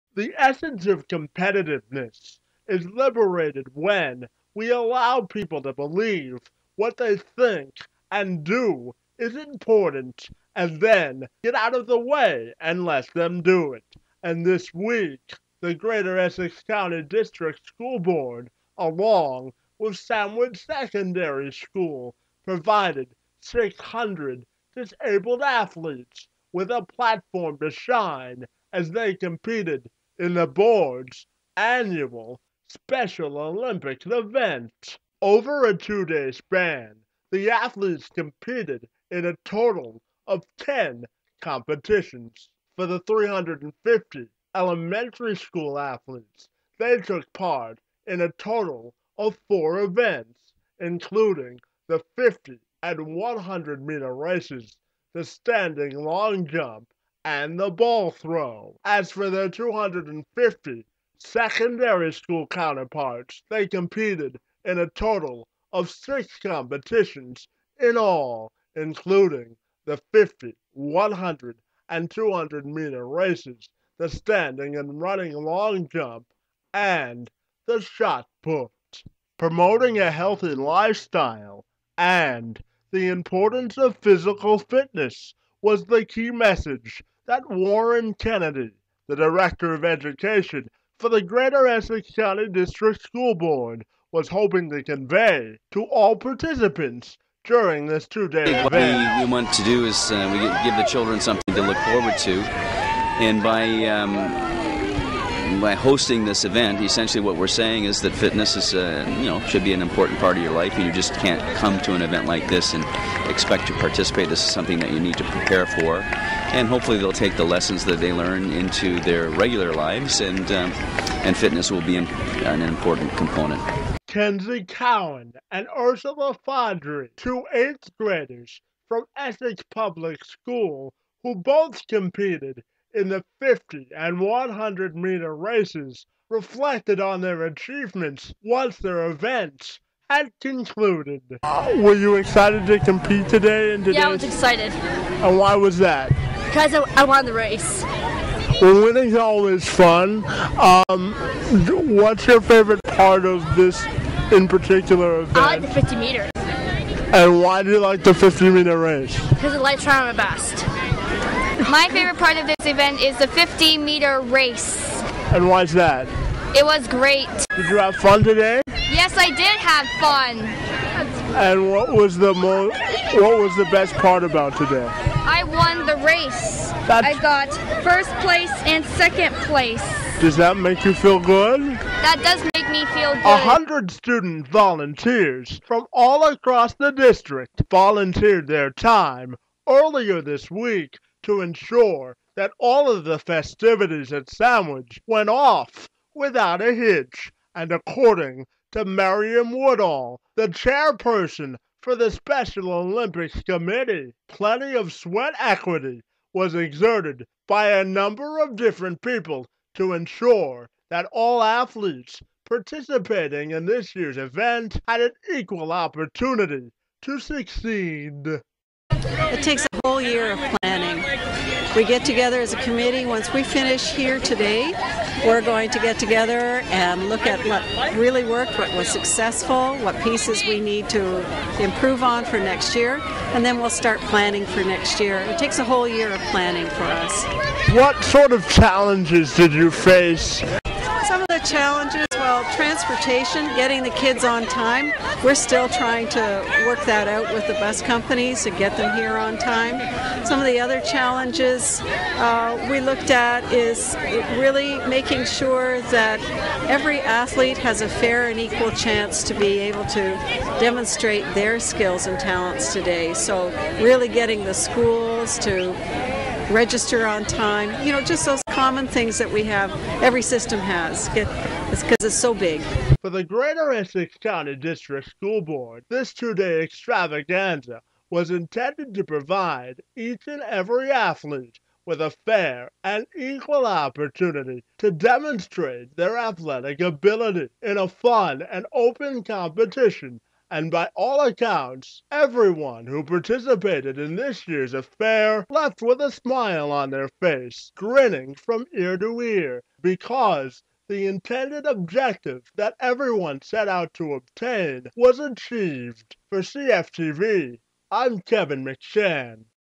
Special Olympics Story
A total of 600 disabled student-athletes from schools across the Greater Essex County District School Board took part in the Board's annual Special Olympics at Sandwich Secondary School in LaSalle May 18th & 19th. With the help of a group of enthusiastic community partners and more than 100 student volunteers, this year's event will be memorable. The elementary school participants will take part in 4 competitions: 50 and 100 metre races, the ball throw and standing long jump. When they arrive at the Carrick Complex in the morning, they'll march in accompanied by the Marlborough and Anderdon Public School Drum Lines.
The next day, the student-athletes from the Board's secondary schools will enjoy the same grand ceremonial opening before they begin a schedule of 6 events: 50, 100 and 200 metre races, shot put and standing and running long jump. The goal of the GECDSB Special Olympics is to provide every student with an equal and fair opportunity to demonstrate their athletic ability in open and fun competition.